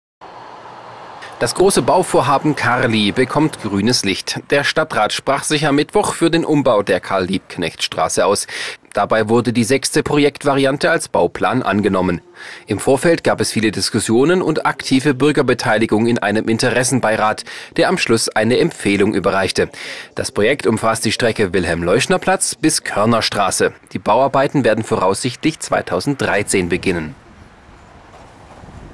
Stadtrat beschließt Planungsvariante 6 für Umbau KARLI (19.04.2012)
Das große Bauvorhaben KARLI bekommt grünes Licht. Der Stadtrat sprach sich am Mittwoch für den Umbau der Karl-Liebknecht-Straße aus. Dabei wurde die 6. Projektvariante als Bauplan angenommen. Im Vorfeld gab es viele Diskussionen und aktive Bürgerbeteiligung in einem Interessenbeirat, der am Schluss eine Empfehlung überreichte. Das Projekt umfasst die Strecke Wilhelm-Leuschner-Platz bis Körnerstraße. Die Bauarbeiten werden voraussichtlich 2013 beginnen.